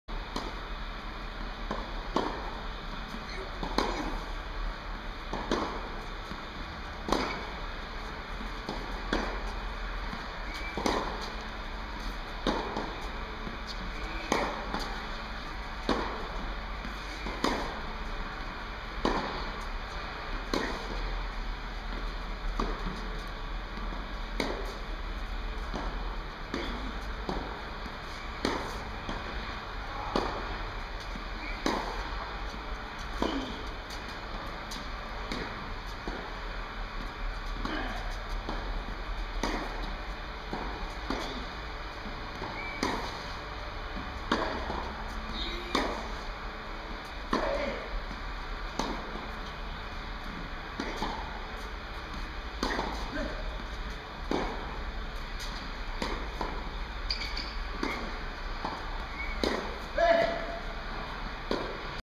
TENNIS DRILL PART 3.avi
This training happened on Friday from 8 to 10. Before the training I took Vita-Min pill, one dose of Pro Mix Protein, 250 mlg pill C vitamin plus 200 mlg aspirin pill.  Then after the tennis training I took a breakfest and at 13.00 gym training. Before the gym training another dose of Pro Mix protein plus one pill of creatin. Coming back from gym I took my lunch, high on proteins, less on carbos. After the lunch I took one pill of creatin, and 2.5 hours after the lunch I took again one dose of Pro Mix protein (whey/casein).